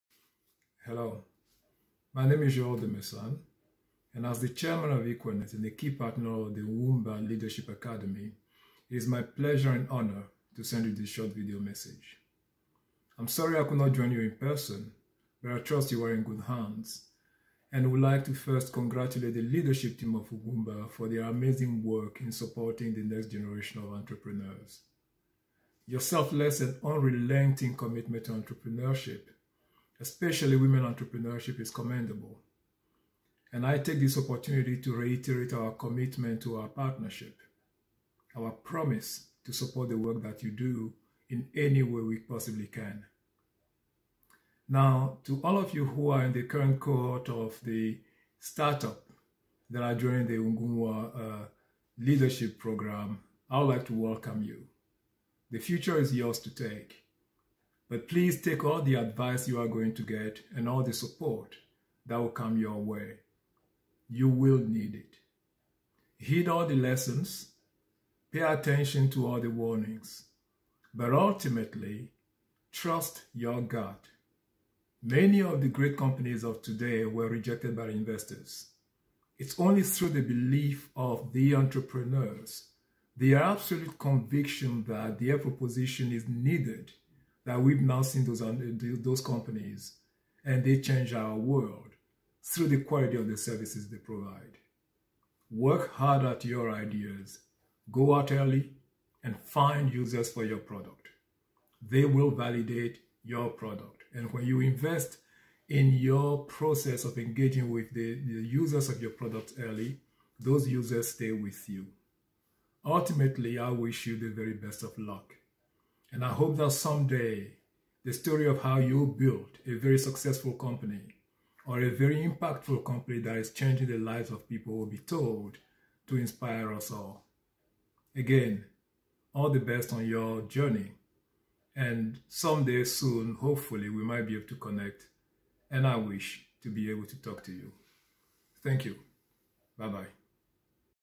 UEC2021: GOODWILL MESSAGE FROM CEO OF EQUANET
The 2021 Ugwumba Enterprise Challenge, ended in grand style with goodwill messages from our partners across the world.

Here in the video is the goodwill message from one of our partners, Joel de Messan, the CEO of Equanet UK.

Click below to listen to what he said about the Ugwumba Enterprise Challenge...